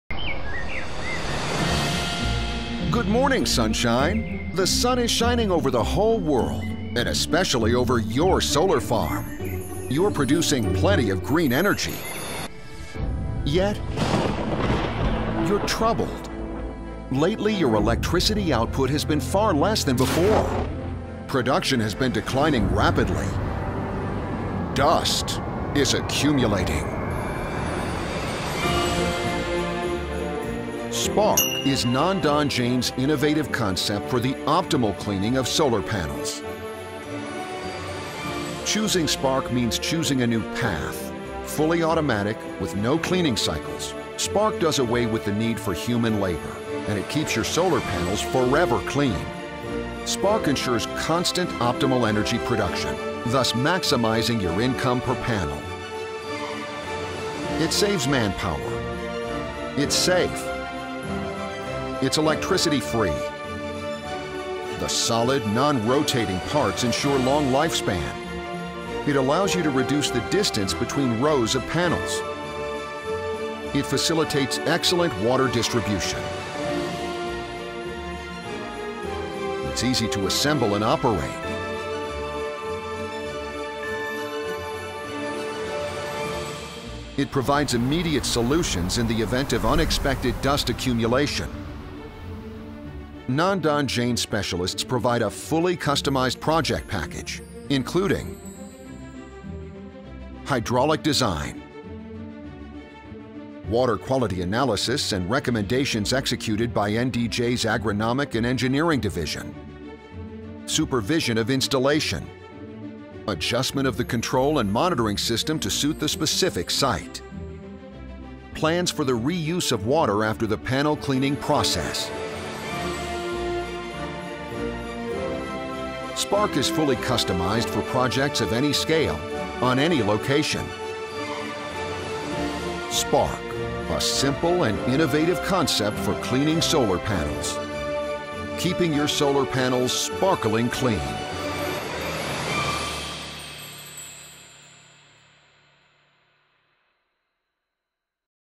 Vệ sinh tấm pin năng lượng mặt trời bằng công nghệ tưới Naandanjain - Green Agritech
Công ty GreenAgritech giới thiệu một phương pháp rất hiệu quả để vệ sinh tấm pin năng lượng mặt trời ứng dụng công nghệ và béc tưới Naandanjain - Nhập khẩu Israel. Không còn phải lau chùi bằng tay, điều khiển hoàn toàn tự động, thích hợp các hệ thống năng lượng mặt trời áp mái gia đình đến các hệ thống điện công nghiệp hàng chục đến hàng trăm héc-ta.
Hiệu quả - An toàn - Hiện đại - Tiết kiệm.